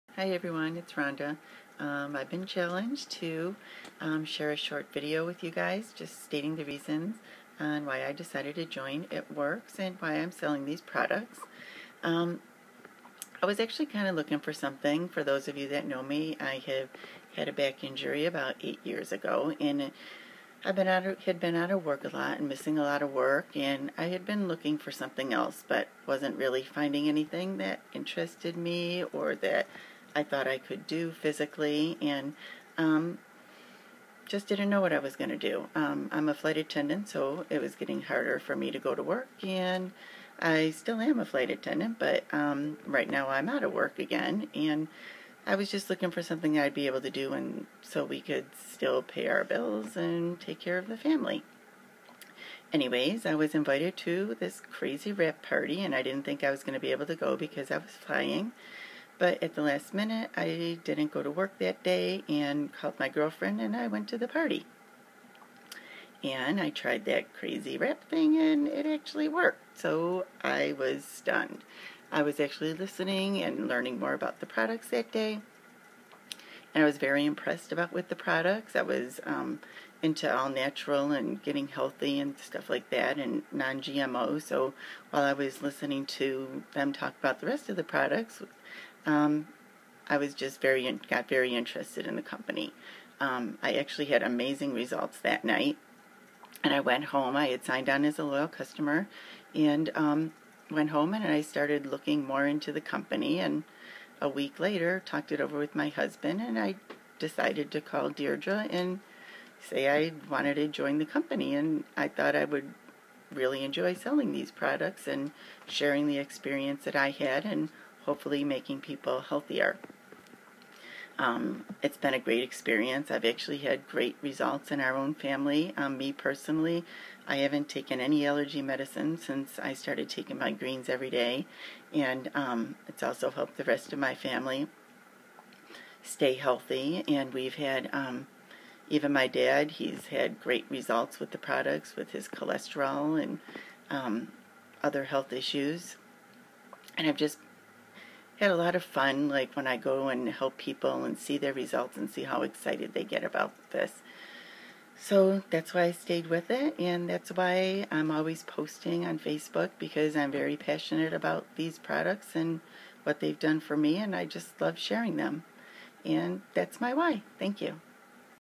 My "WHY" for joining It Works Global.
Here is a short video on why I joined It Works and why I love sharing these products!  Hope you enjoy it.